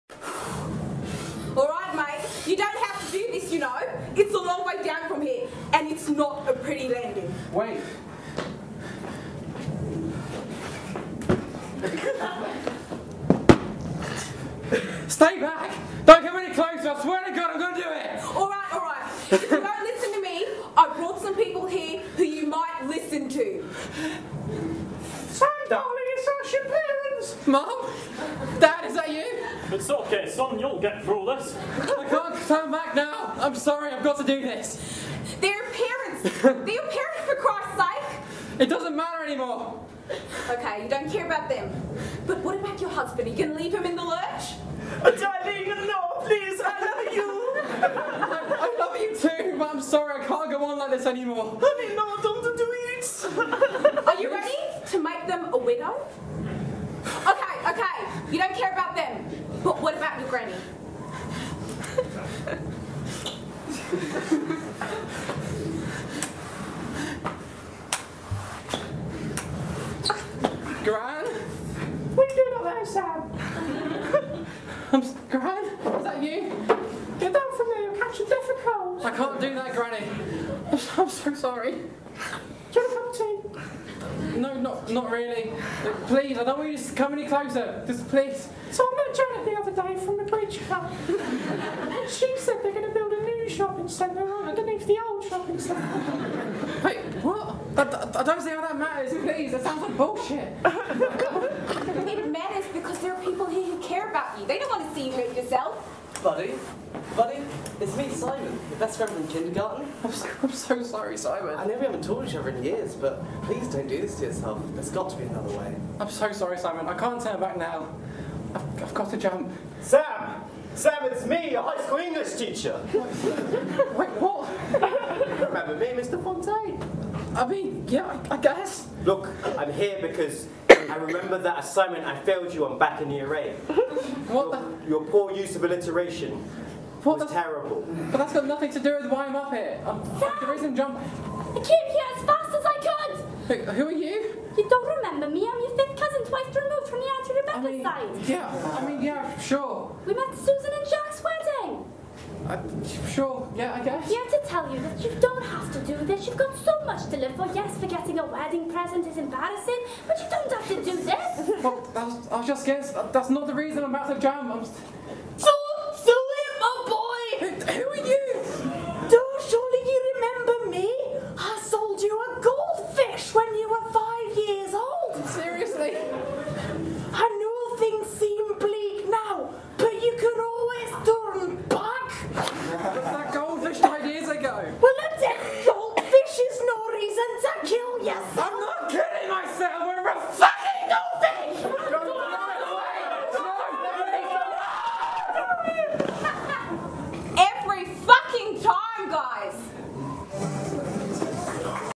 The Jumper - [Caution! Dry Ceiling]
The Jumper from the University of Southampton's Comedy Society's Freshers' Sketch and Improv Show 2018, [Caution! Dry Ceiling].

Written by Ciaran Madeley
Starring Sam Foster, Sharon Wanjohi, Mirek Gosney, Sam Barry, Ed Maynard, Ted Woosley, Ash Subramanian, Maddy Rees, & Abbie Edwards.

Filmed by Stephen Potter on 09/11/18